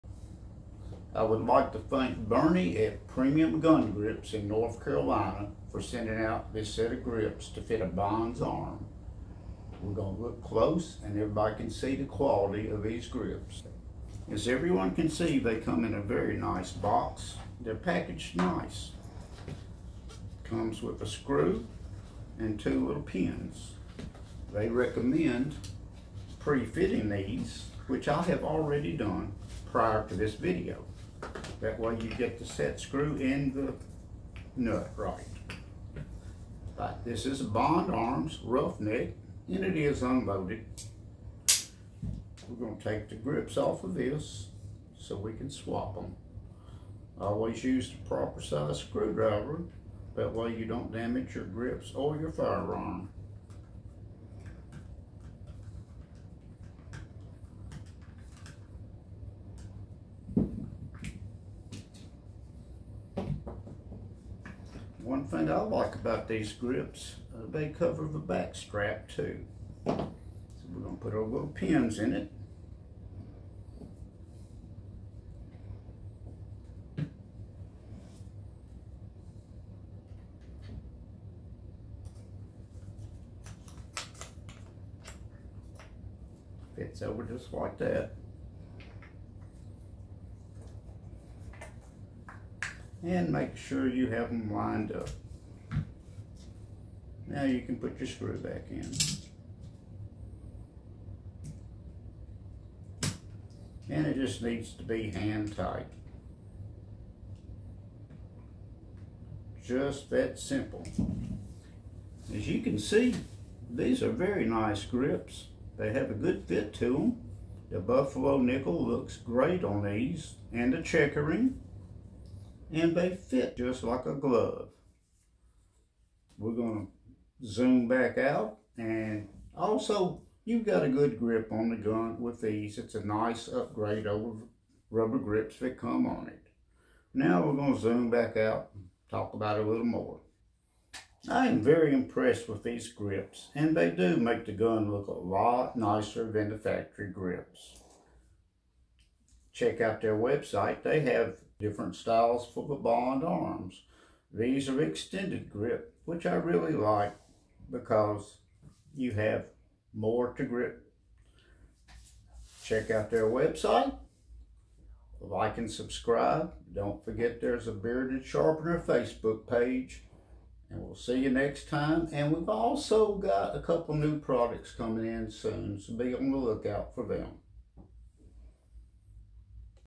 BUFFALO NICKEL GRIPS - BOND ARMS
This good looking set of Rosewood stained hardwood grips are one of many custom grips from PREMIUM GUN GRIPS. NOTE, Fitting the grips together off the gun allows the screw nuts to seat better.They have an excellent fit and finish. Tell Bernie I sent you. This set will give you more real estate on this firearm and give it a classier look of a more expensive firearm.